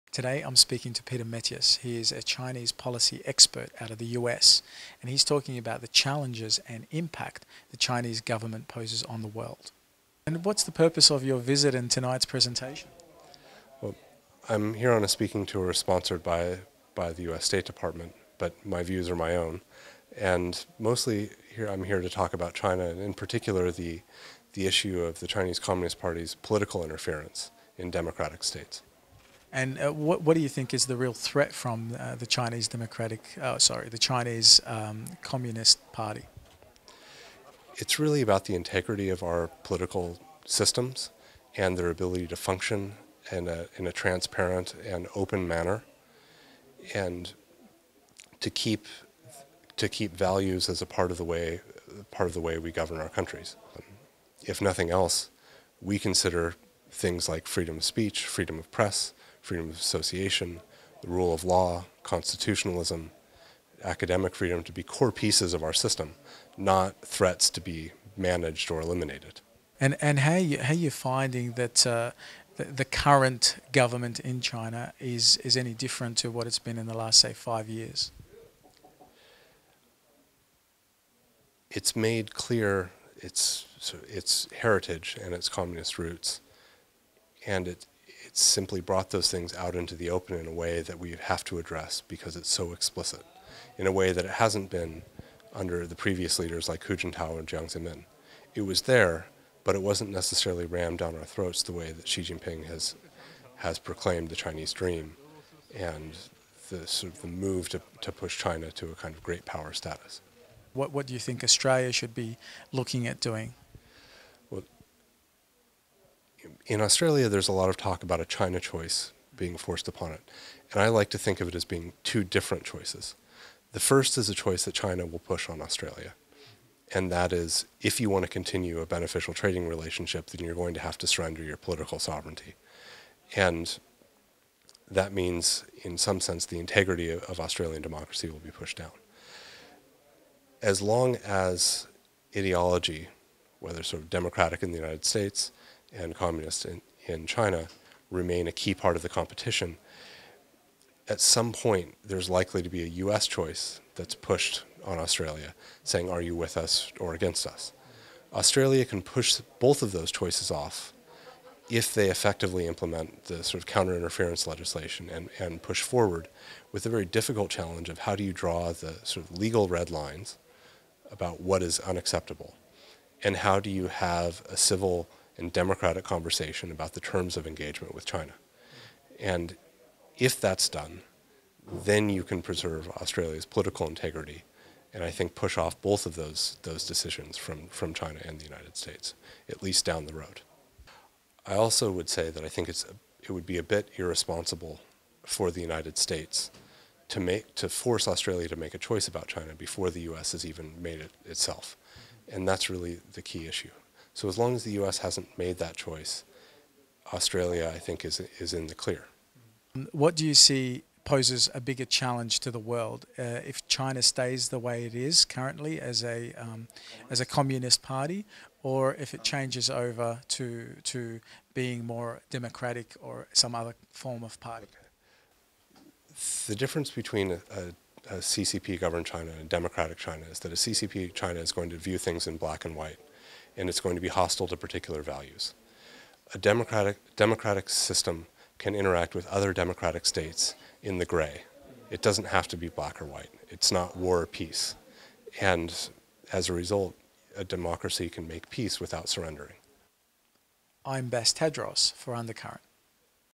WTV UCS25EP04 Peter Mattis On China
Is China a threat to the world given they have a communist government?

Watch this episode of WTV, Peter Mattis is interviews by Bass Tadros on this topic.